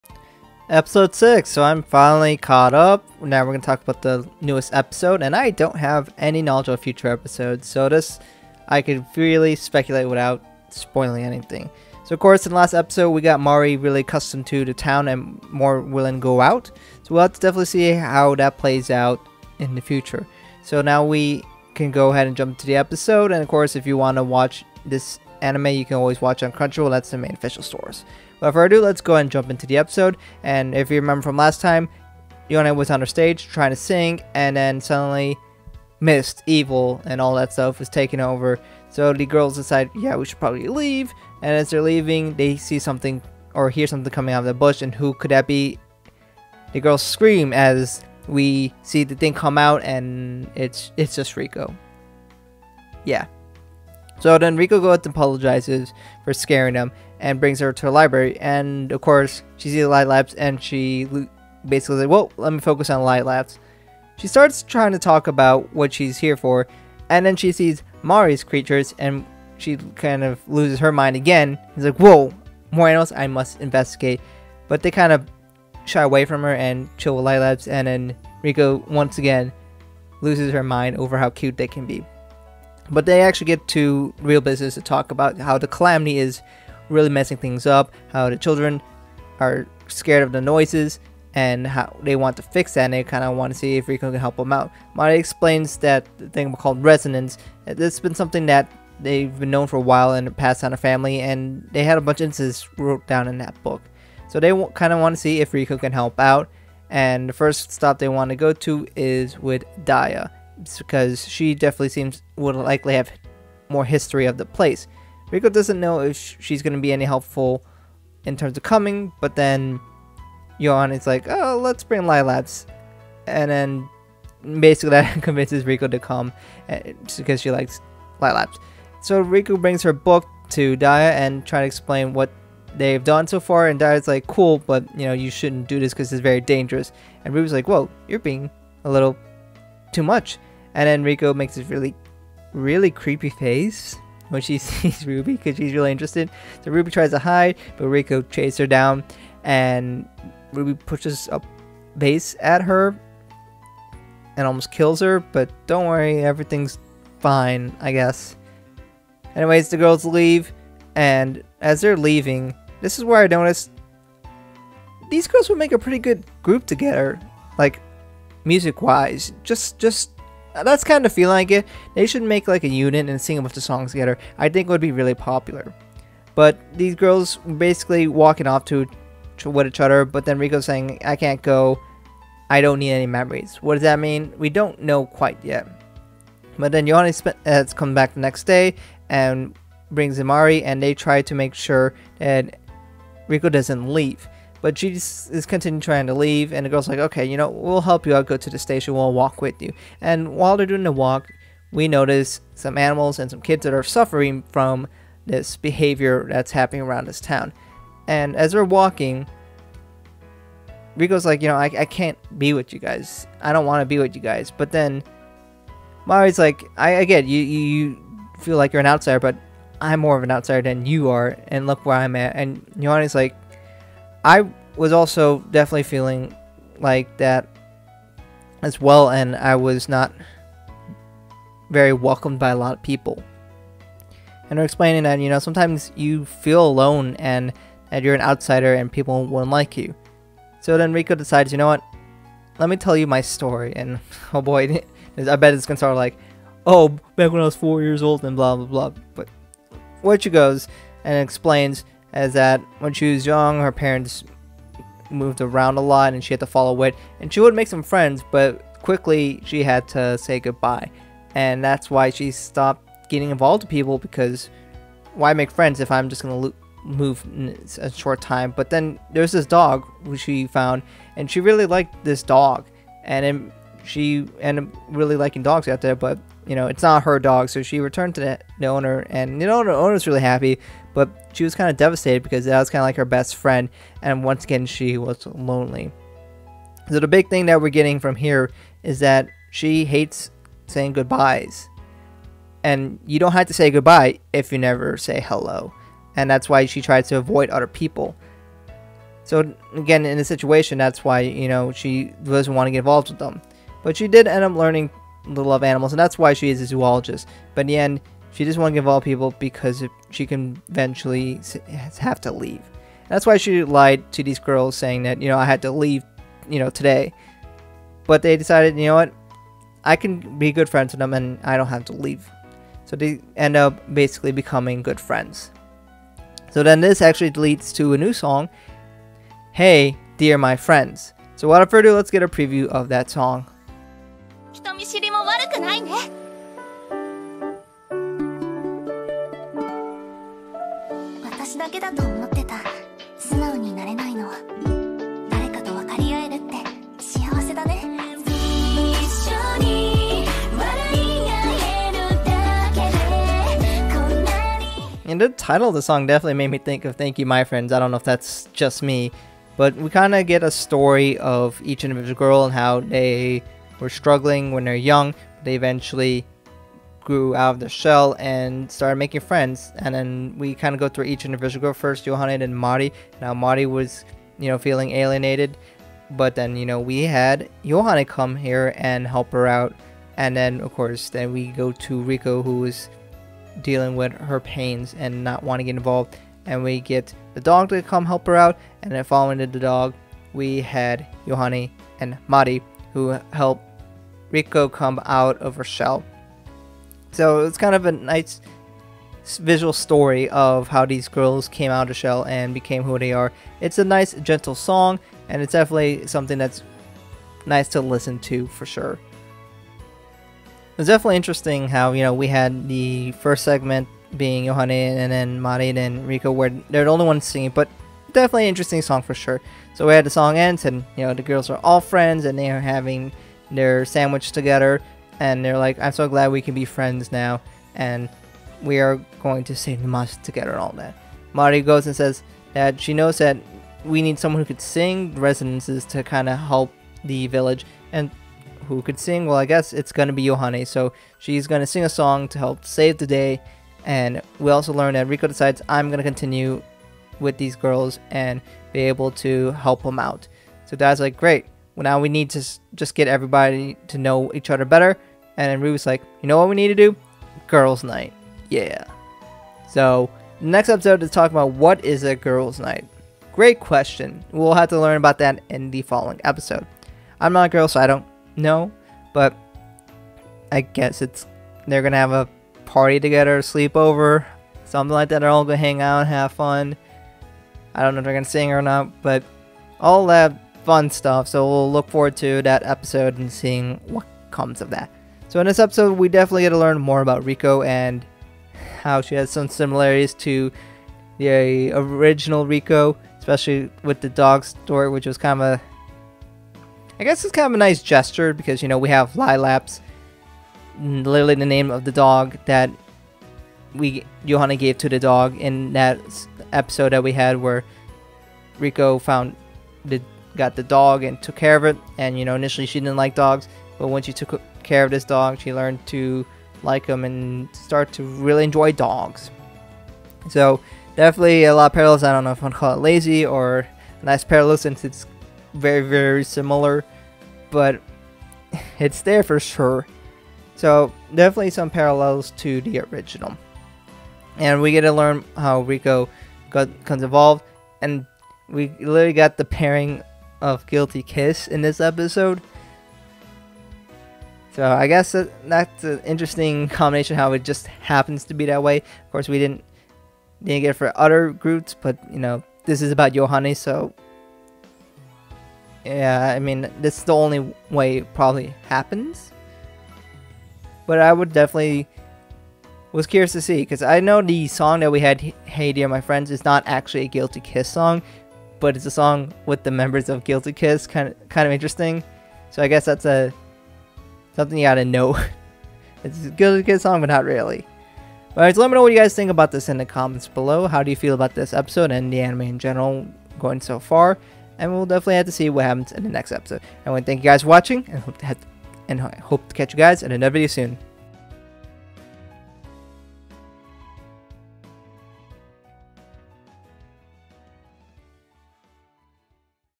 Genjitsu no Yohane S1 E06 Review
This review goes over the sixth episode of the Yohanime (Yohane of the Parhelion). Yohane now needs to get Riko to help out. Riko is afraid to make friends, but will Yohane be different?